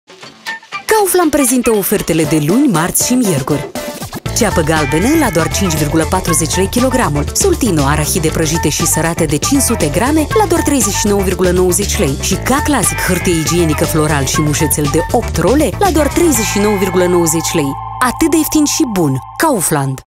Oferta Kaufland din 2-4 octombrie: ceapă galbenă, arahide și hârtie igienică
Kaufland prezintă ofertele de luni-miercuri!

Între 2 și 4 octombrie ai:

- ceapă galbenă la doar 5,40 lei/kg;
- arahide prăjite și sărate la doar 39,90 lei/500 g;
- hârtie igienică, 8 role, la doar 39,90 lei.